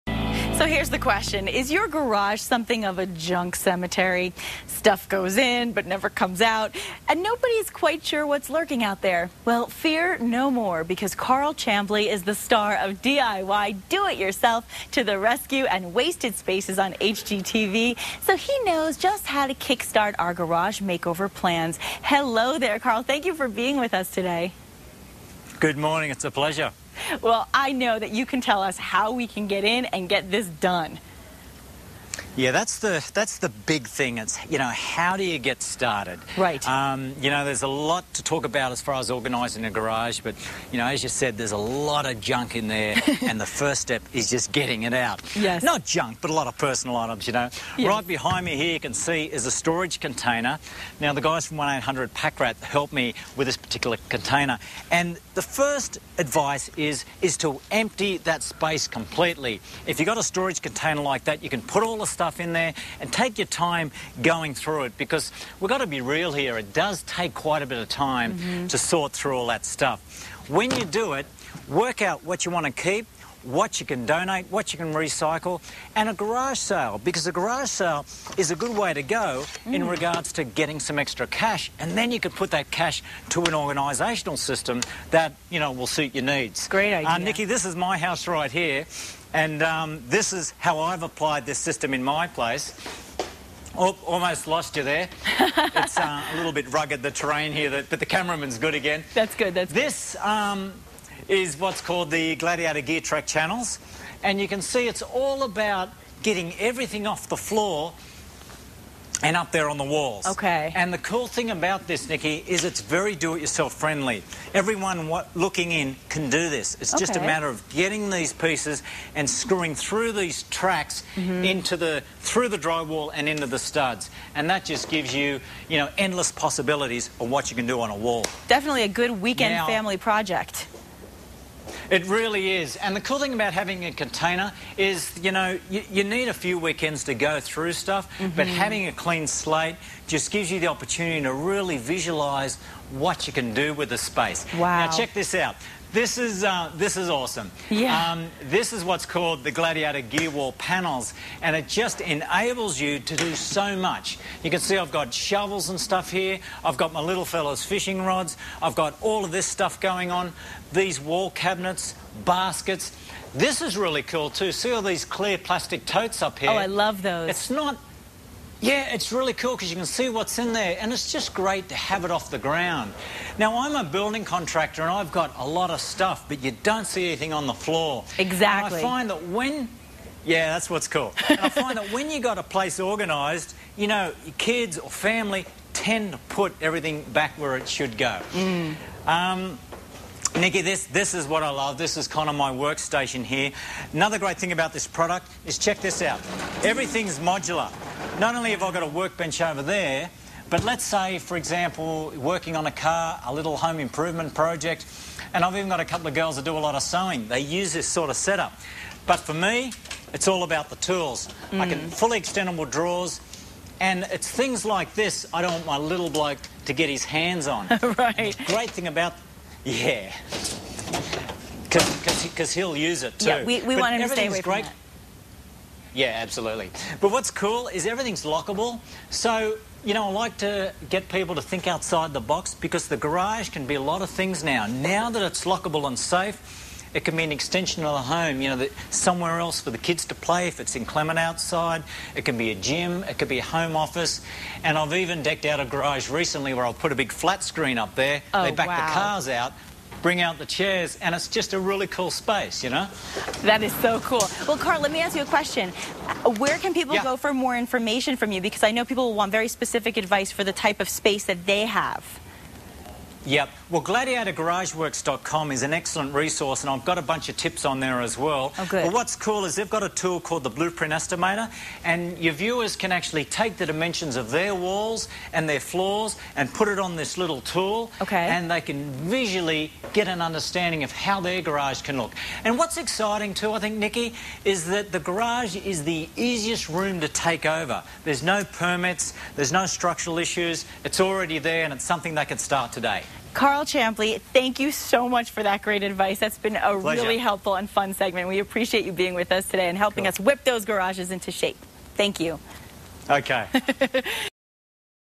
DIY Garage Cleaning
Karl Champley, Home Improvement Expert and Star of "DIY (Do It Yourself) to the Rescue" and "Wasted Spaces," on HGTV, joins Nikki from Los Angeles with tips for Garage Makeover Plans.